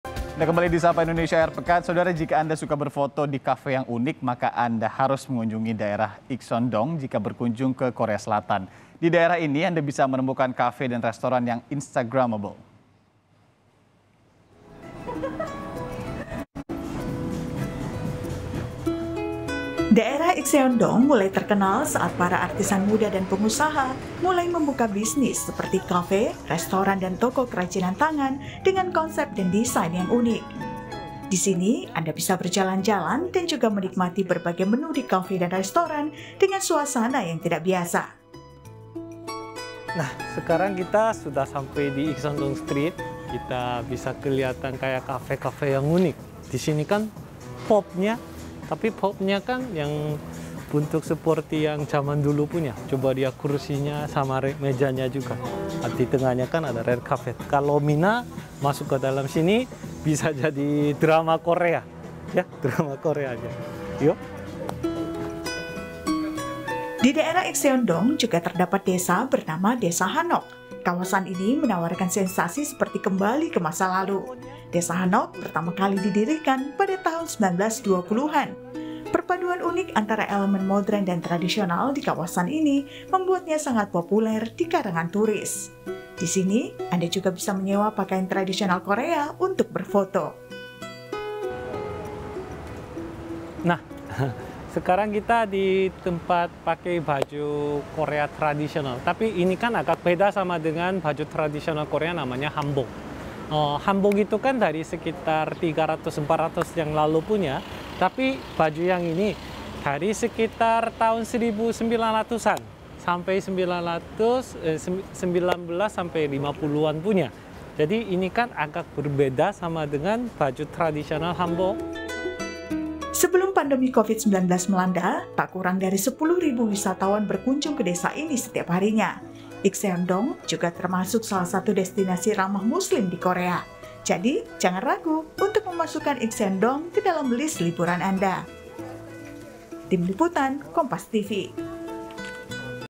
Uniknya Kawasan Ikseon-dong, Pertemuan Elemen Modern dan Tradisional di Korea Selatan!
KOREA SELATAN, KOMPAS.TV - Daerah Ikseon-dong mulai terkenal saat para artisan muda dan pengusaha mulai membuka bisnis seperti kafe, restoran, dan toko kerajinan tangan dengan konsep dan desain yang unik.

Di sini anda bisa berjalan-jalan dan juga menikmati berbagai menu di kafe dan restoran dengan suasana yang tidak biasa.

Baca Juga Insa-dong Penuh Keindahan Seni Tradisional, Jadi Destinasi Wisata Ramah Muslim di Korea Selatan di

Di daerah Ikseon-dong ini juga terdapat desa bernama Desa Hanok.

Kawasan ini menawarkan sensasi seperti kembali ke masa lalu.

Desa Hanok pertama kali didirikan pada tahun 1920-an.

Perpaduan unik antara elemen modern dan tradisional di kawasan ini membuatnya sangat populer di kalangan turis.

Di sini, anda juga bisa menyewa pakaian tradisional Korea untuk berfoto.

Sebelum pandemi covid-19 melanda, tak kurang dari 10.000 wisatawan berkunjung ke desa ini setiap harinya.

Ikseon-dong juga sudah termasuk salah satu destinasi ramah muslim di korea.

Jadi jangan ragu untuk memasukkan Ikseon-dong ke dalam list liburan anda.